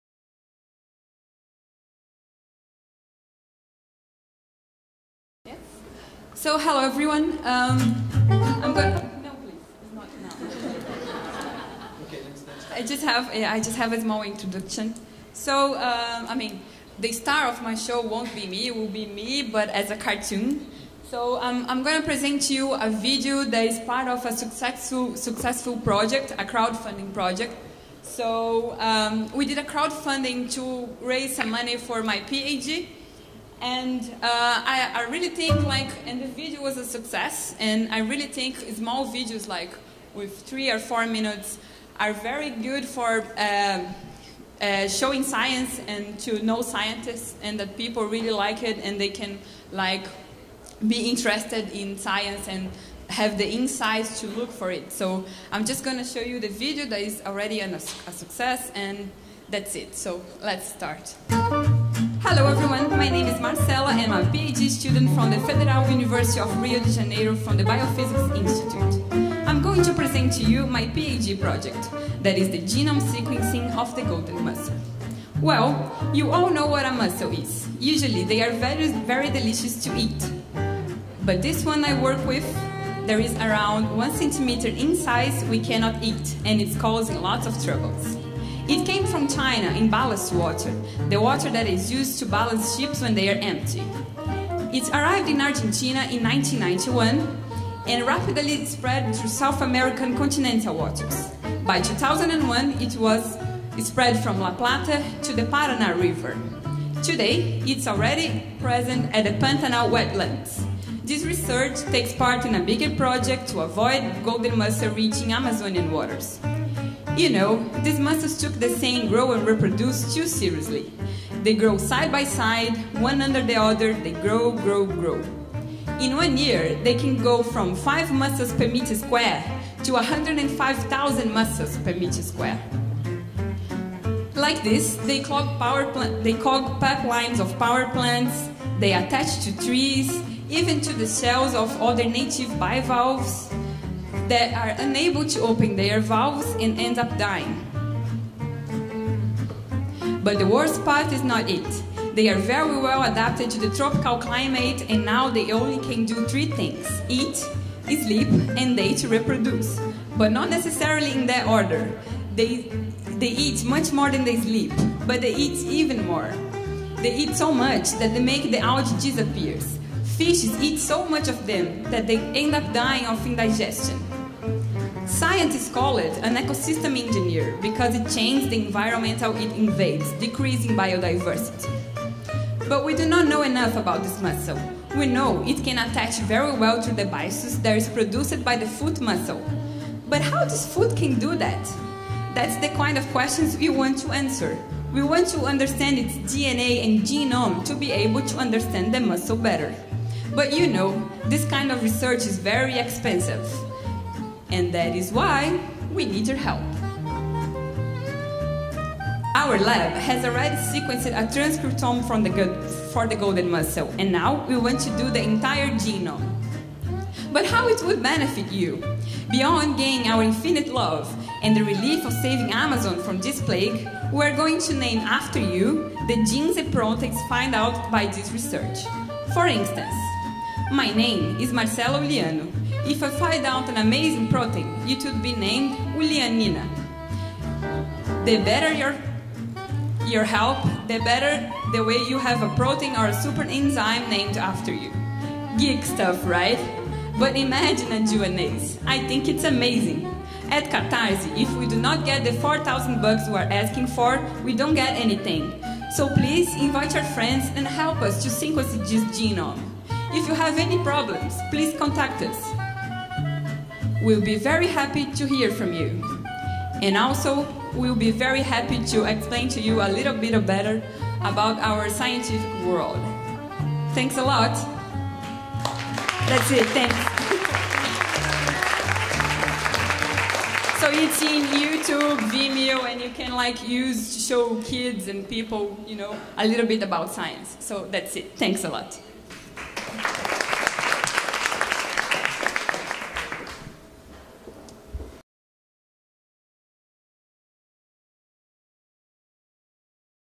Euraxess Science Slam Brazil 2013 - Marcela Uliano (finalist)
Presentation of Marcela Uliano, PhD student from the Federal University of Rio de Janeiro (IBCCF / UFRJ), finalist of the 1st Euraxess Science Slam Brazil, held on 24th September 2013, in Rio de Janeiro, RJ, Brazil.
She screened an animated cartoon on the genome sequencing of the golden mussel.